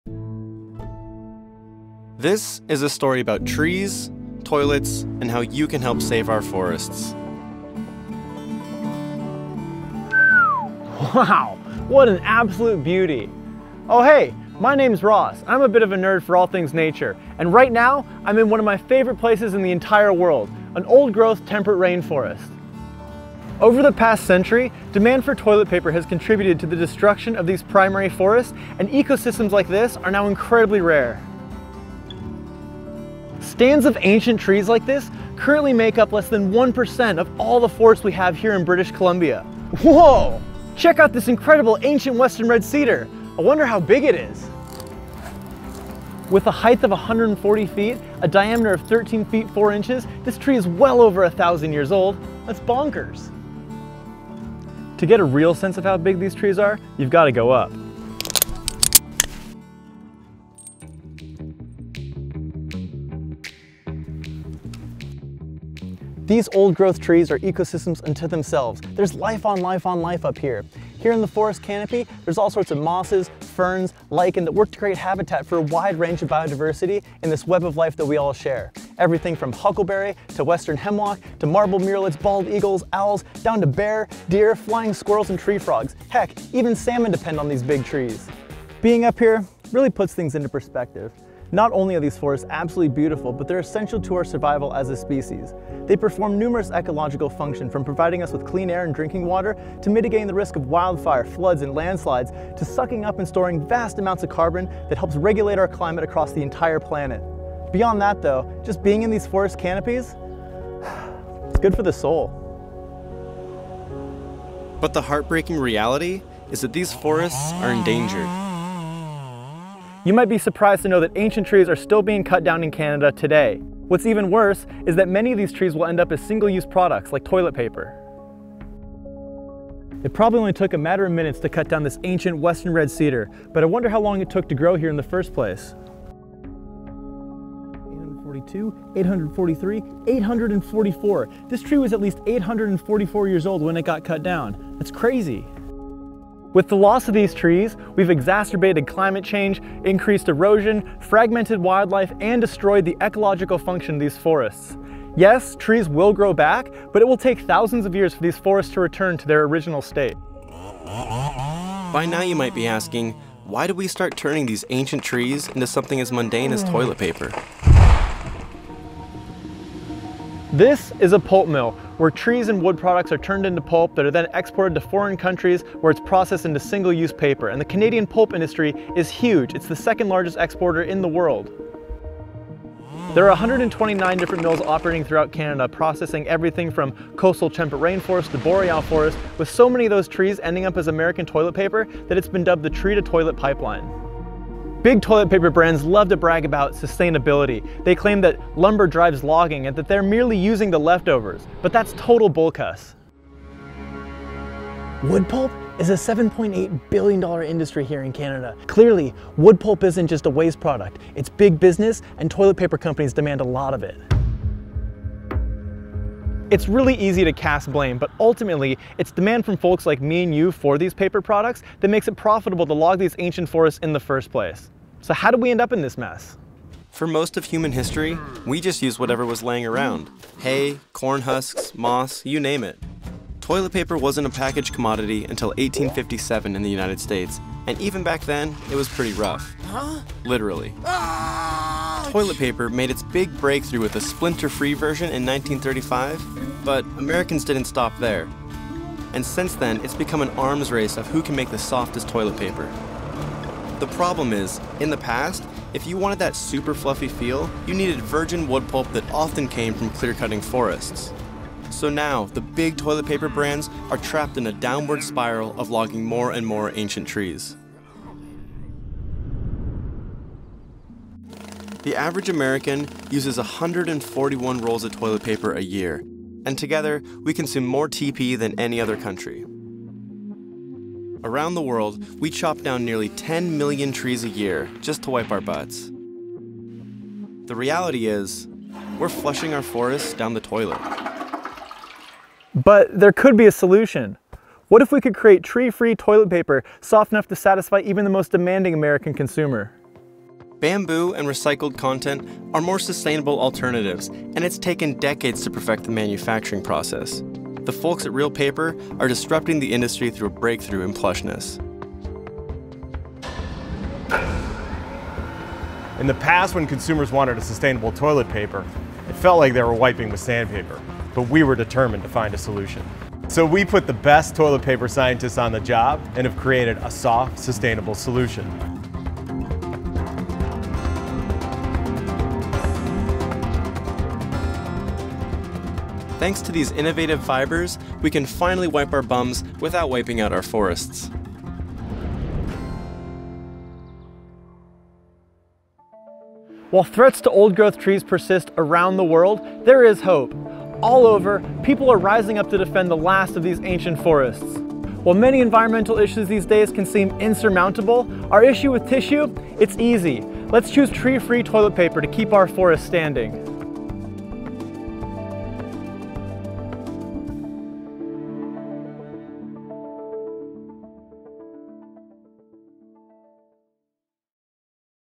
Issue with Tissue - toilet paper's big problem & the sustainable future.
We teamed up with Ross at Nerdy About Nature to share a little bit about why we’re determined to make sustainable, tree-free paper the future of paper. Join us on this fun adventure that takes you through the history of toilet paper, its impact on forests and our planet, and the sustainable path forward being ushered in by sustainable communities around the world.